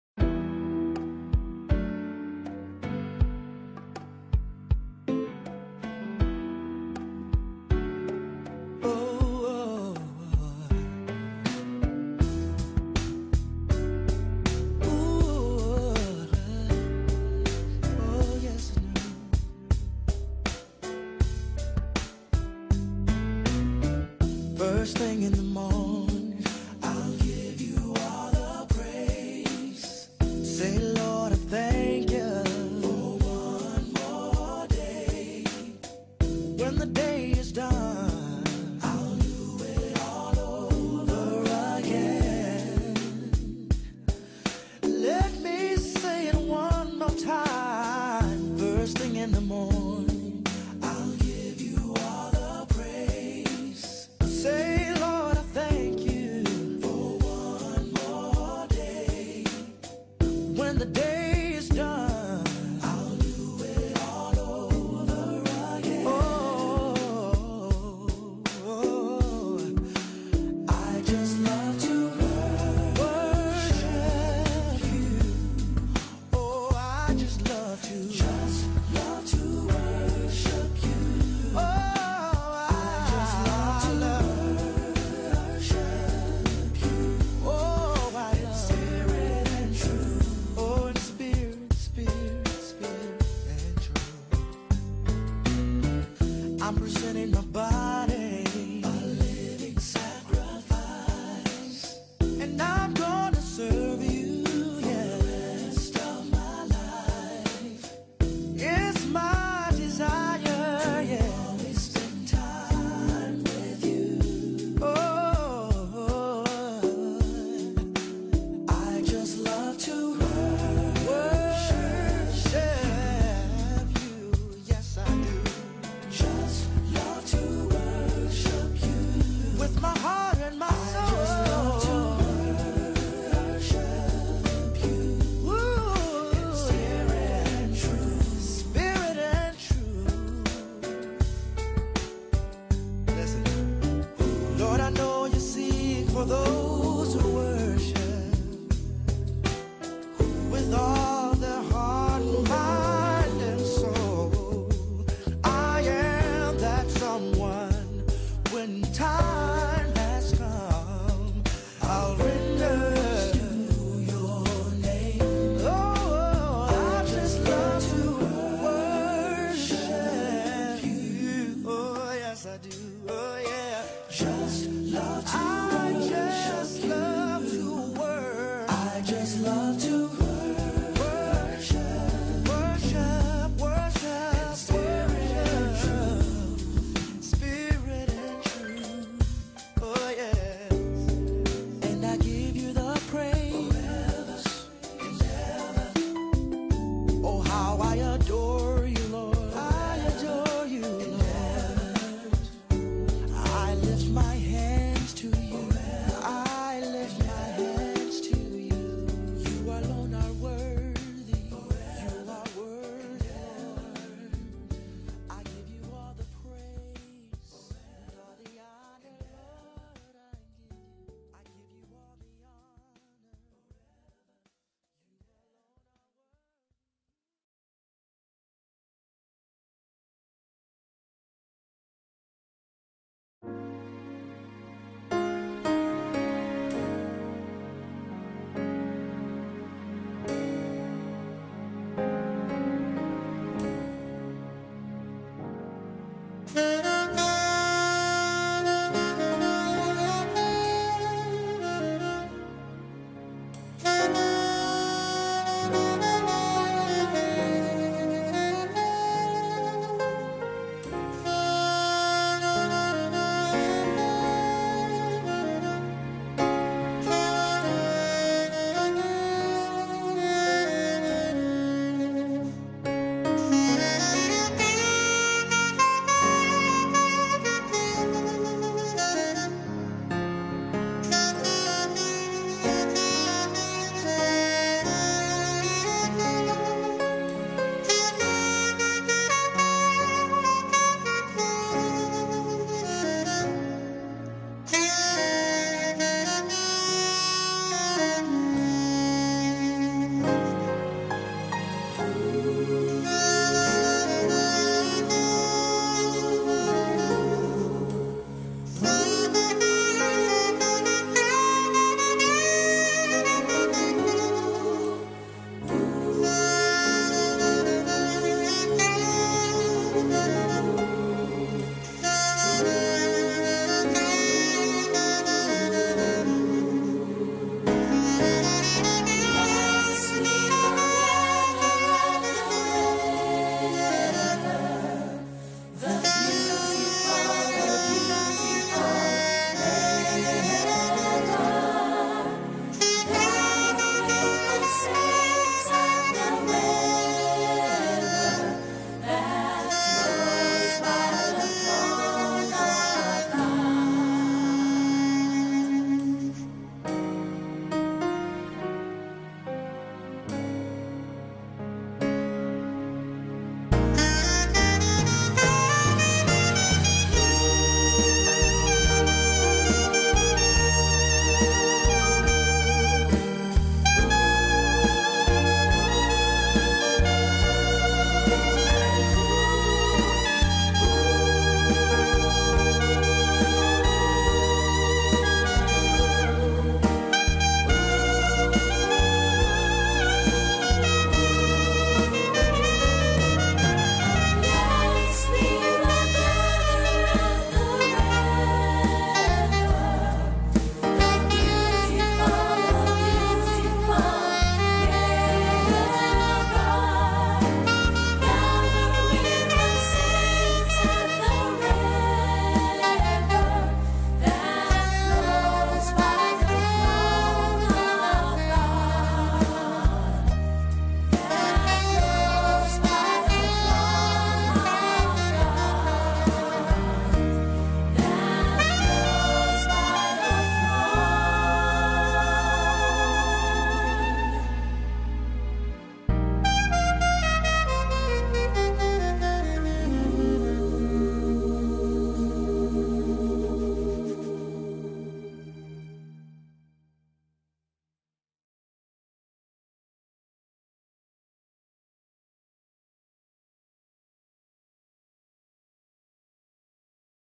Graveside memories
Just posting some of the memorable scenes taken at Juan's committal. it can serve as  memories for some and an opportunity for others to gain some of what they've missed. Pleasant viewing.